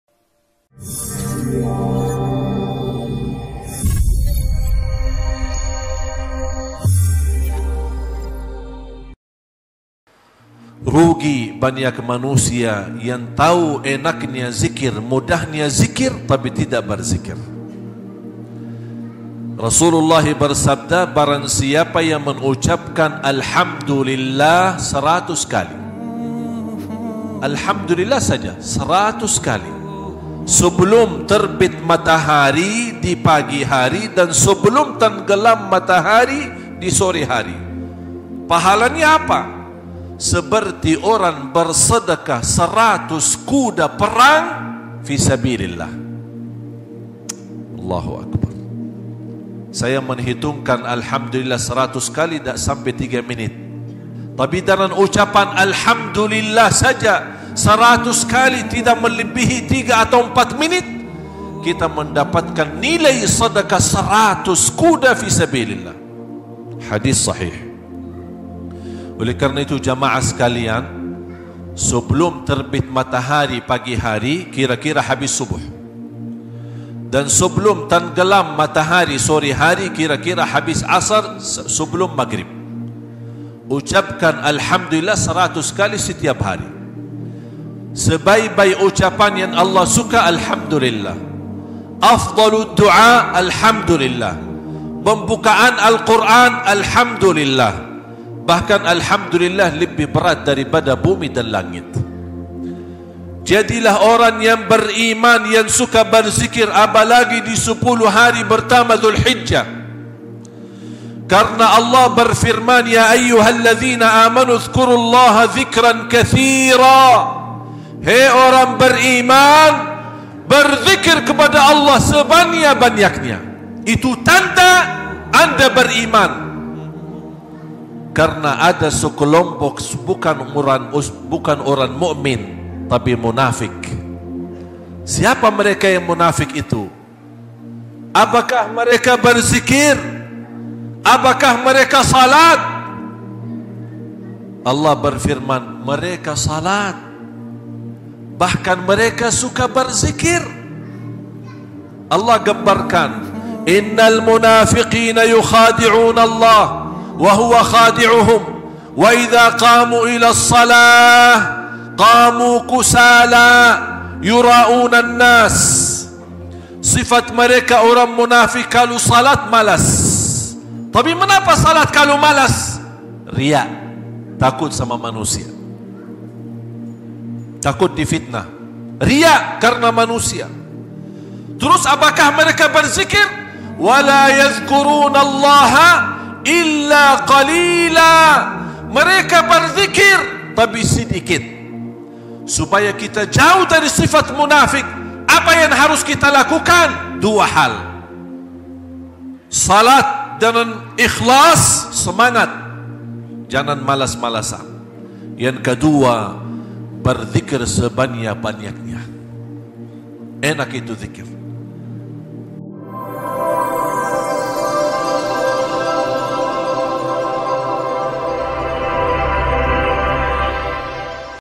Sedekah 100 Kuda Perang Setiap Hari.
Begitu mudahnya, mendapatkan pahala, setara pahala orang bersedekah 100 kuda perang. Dzikrullah setiap pagi dan petang.